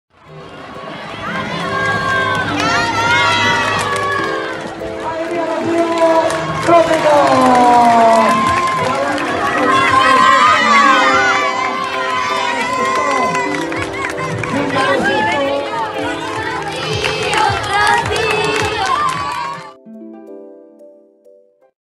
Las chicas de oro DíaDelColegioSPP 10-mayo-19
"Las chicas de oro"
El pasado viernes nuestras chicas de 6° de Primaria consiguieron hacer de la carrera de cross, la más bonita de las carreras vividas hasta el momento. Medalla de oro para todas ellas, porque no sólo gana el que llega primero. ¡Enhorabuena Rocío!